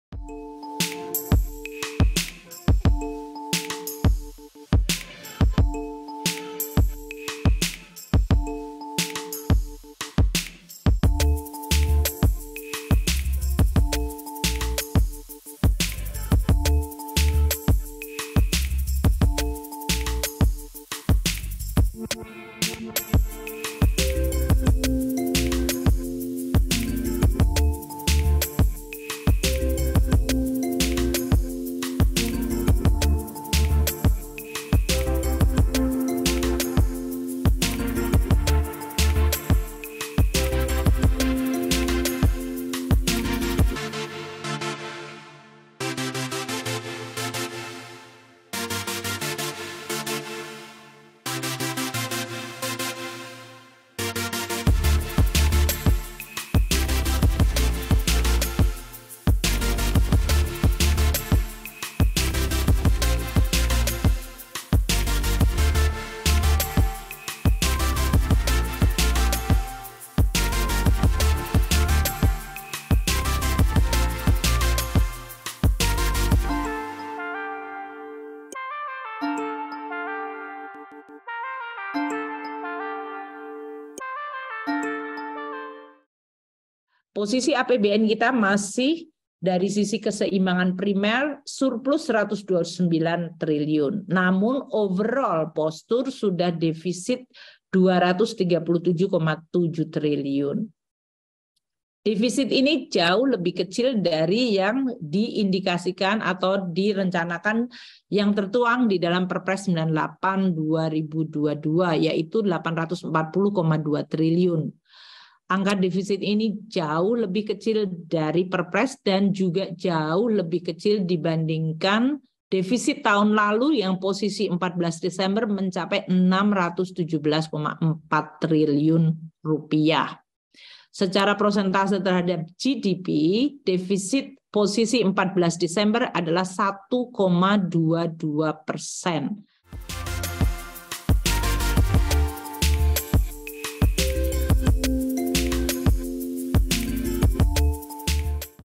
Sri Mulyani Sebut Defisit APBN 1,22% Per 14 Desember 2022
Meski besar pasak dari tiang, APBN 2022 mencetak defisit anggaran yang cukup rendah, yakni 1,22 persen terhadap produk domestik bruto PDB.

Kementerian Keuangan mengklaim, ini menjadi sinyal positif bagi keuangan negara, karena lebih kecil dari yang diindikasikan pada Perpres 98 Tahun 2022.